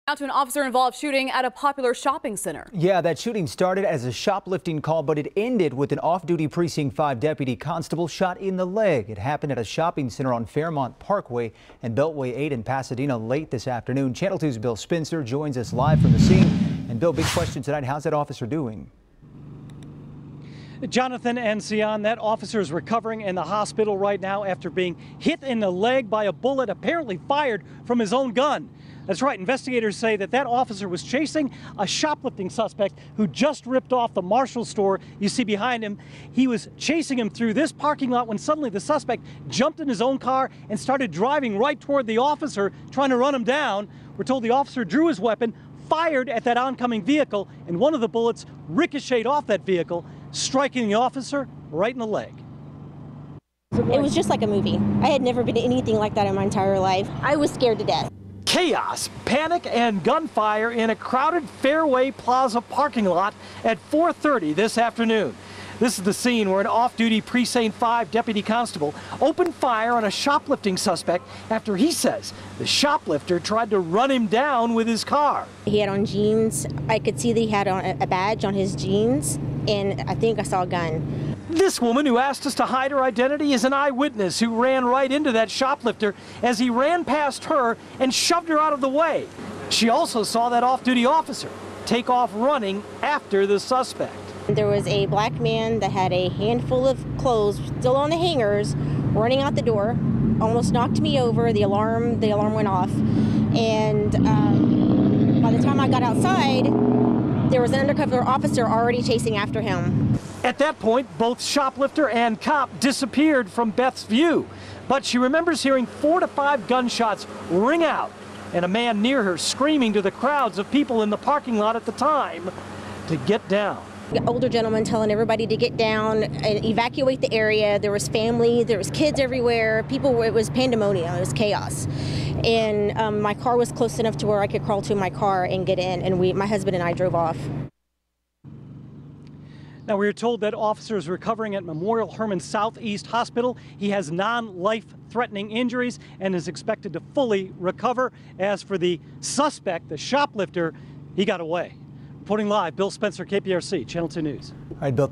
Off-duty deputy constable shot in leg
Constable shot in leg while shop lifter escapes.